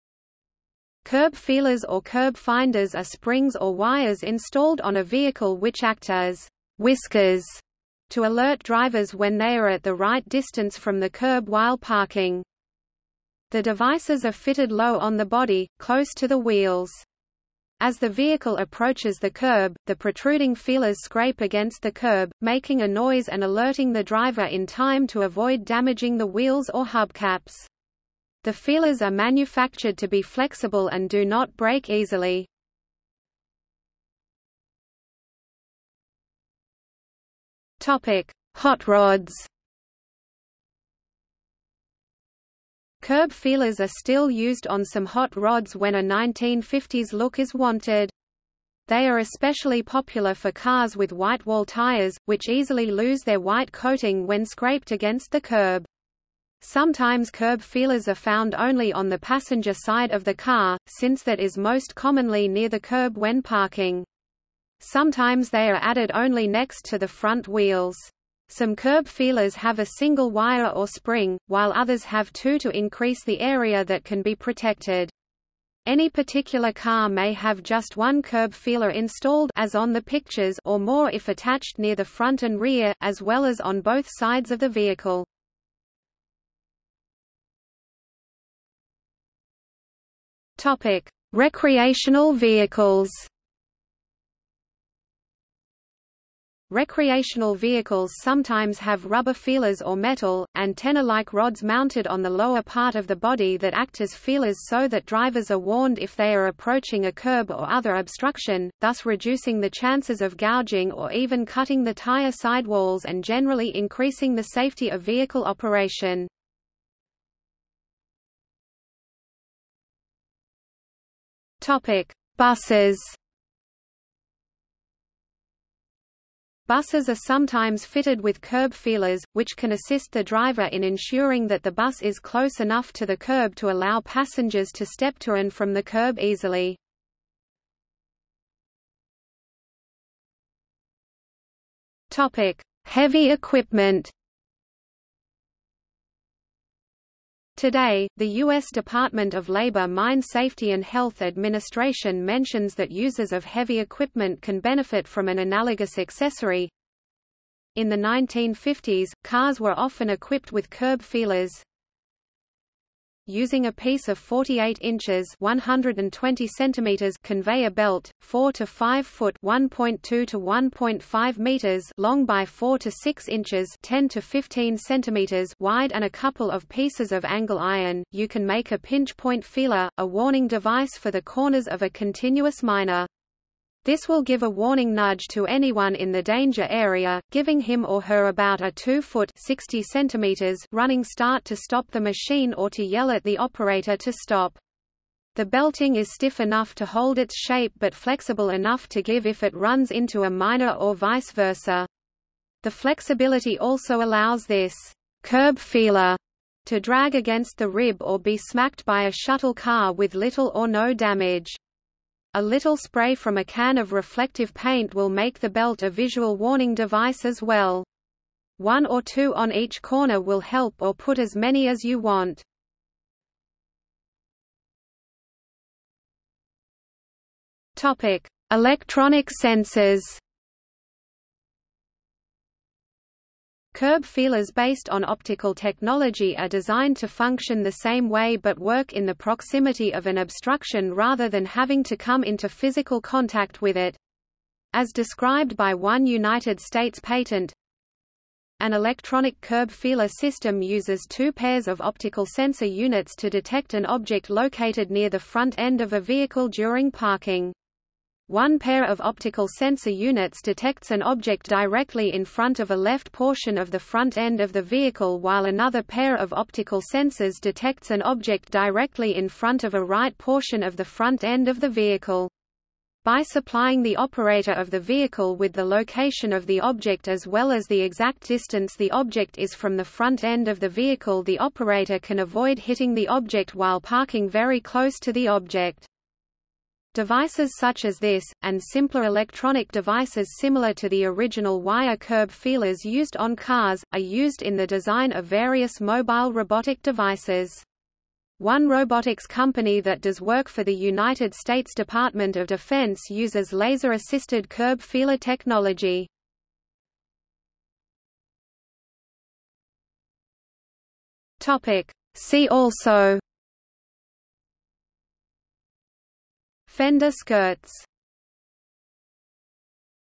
Curb feeler | Wikipedia audio article
00:00:34 1 Hotrods
00:01:27 2 Recreational vehicles
00:01:58 3 Buses
00:02:18 4 Heavy equipment
00:03:44 5 Electronic sensors
00:05:07 6 See also

- Socrates

SUMMARY
Curb feelers or curb finders are springs or wires installed on a vehicle which act as "whiskers" to alert drivers when they are at the right distance from the curb while parking.
The devices are fitted low on the body, close to the wheels. As the vehicle approaches the curb, the protruding feelers scrape against the curb, making a noise and alerting the driver in time to avoid damaging the wheels or hubcaps. The feelers are manufactured to be flexible and do not break easily.